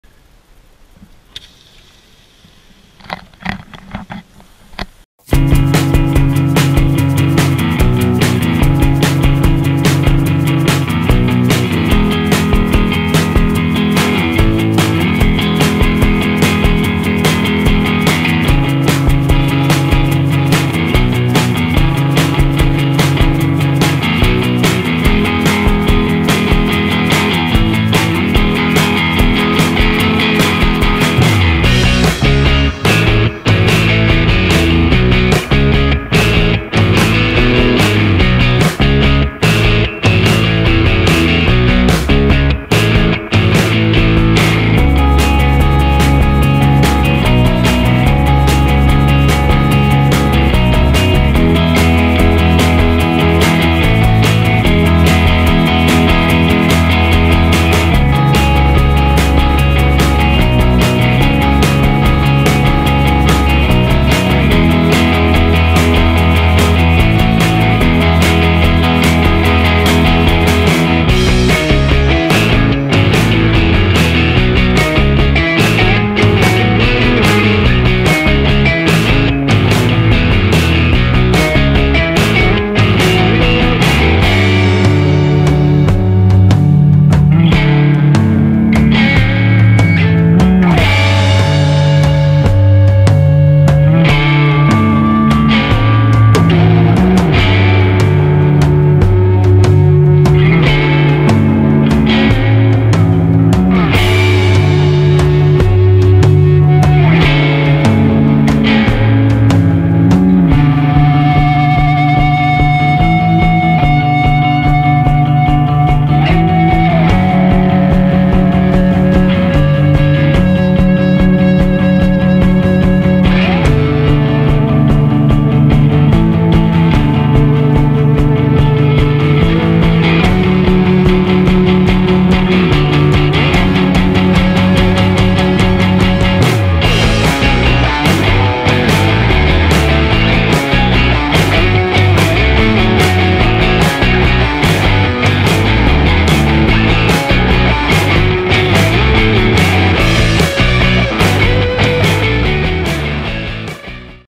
Mystery Beaches
A bit of tomfoolery on a some of the finest hidden NSW beaches. Who knows where these beaches are?